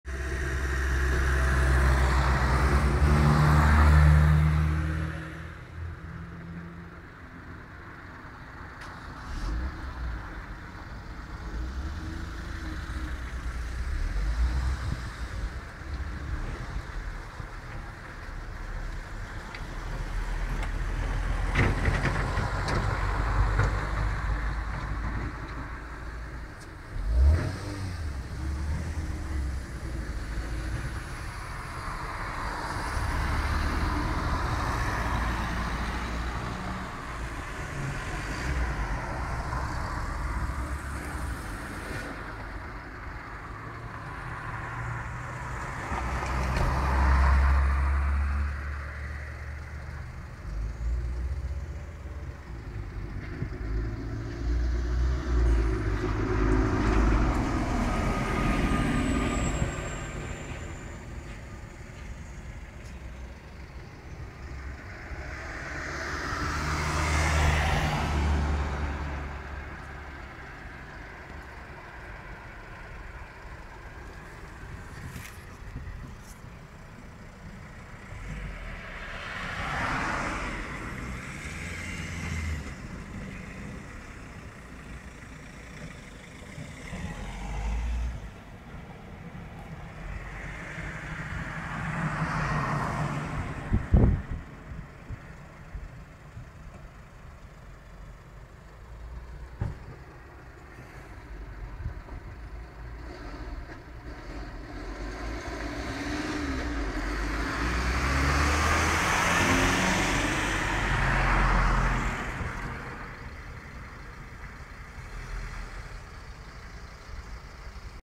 Křižovatka ulic Dvorská , Cihlářská K Meteoru v 7 hod ráno